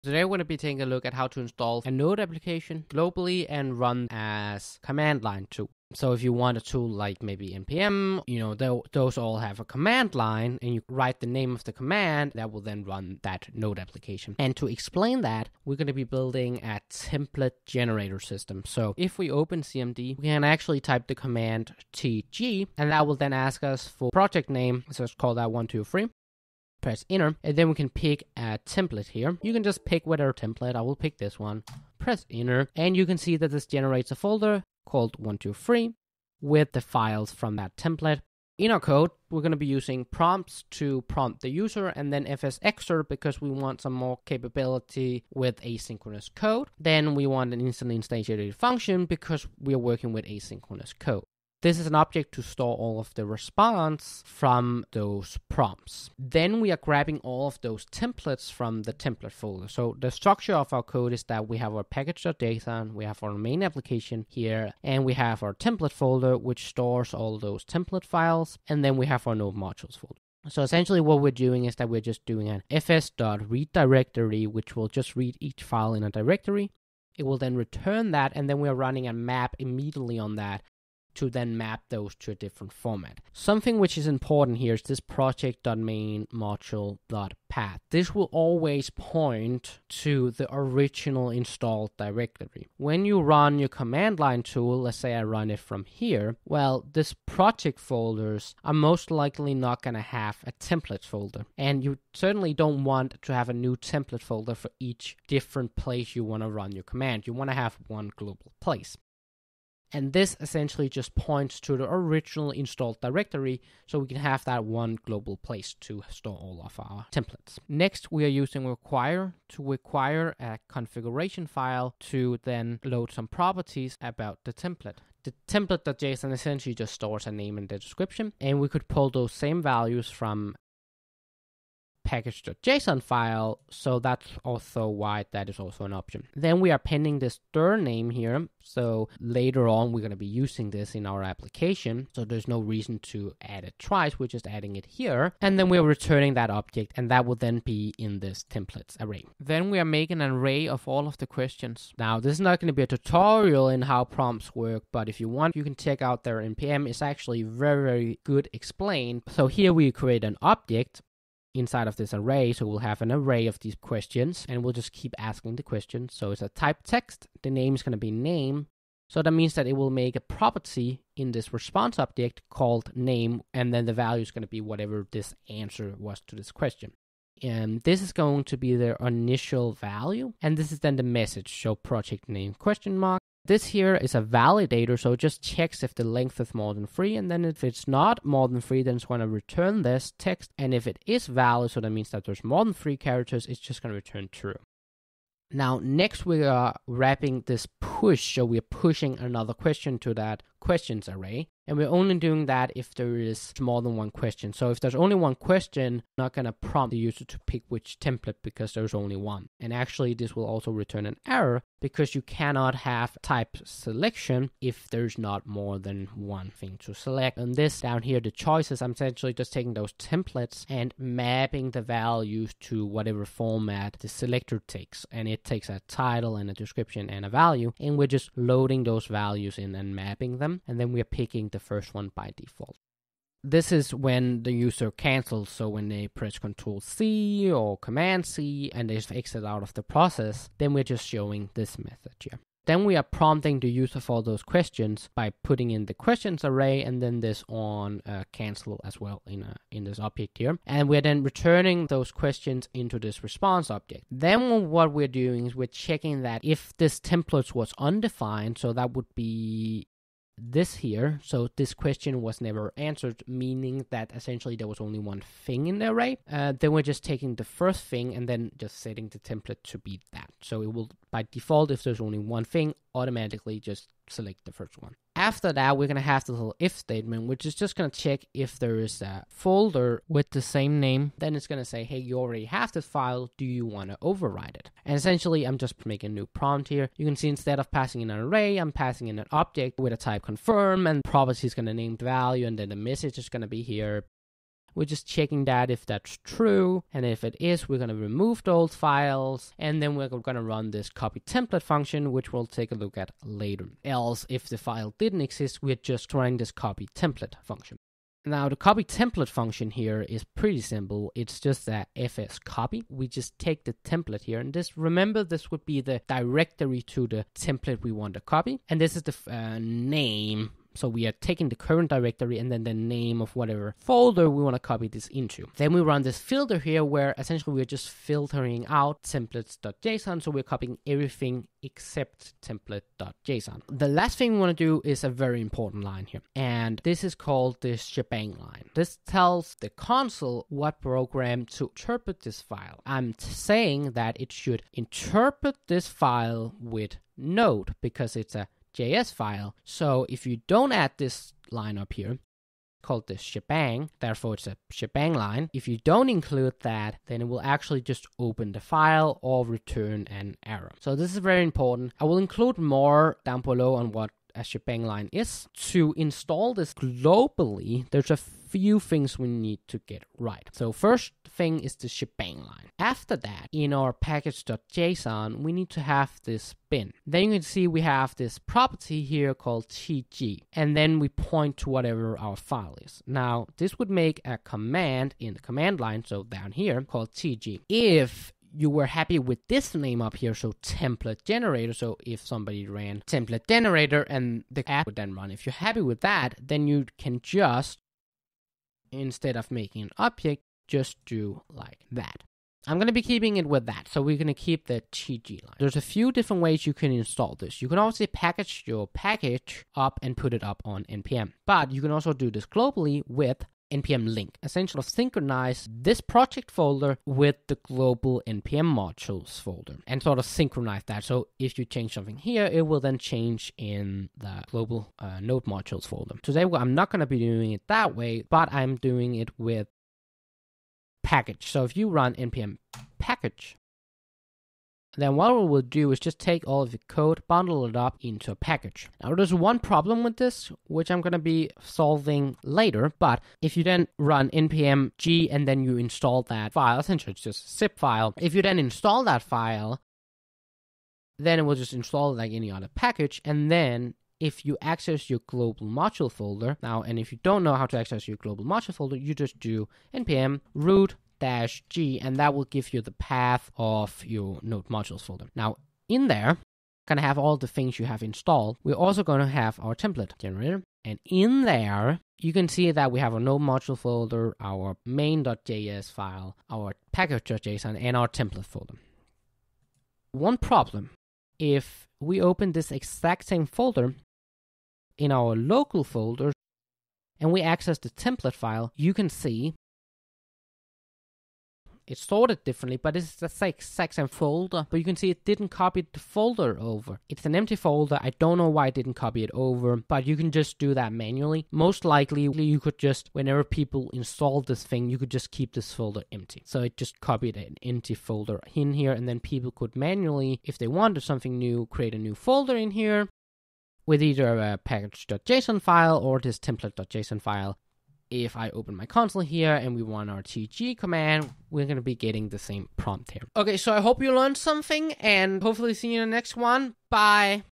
How to Create a Command Line (CLI) Tool in Node.js
In this video I will show you how to turn a Node.js application into a command line tool application

✅ Information about me:

Name: Vincent
Country: Denmark
Age: 21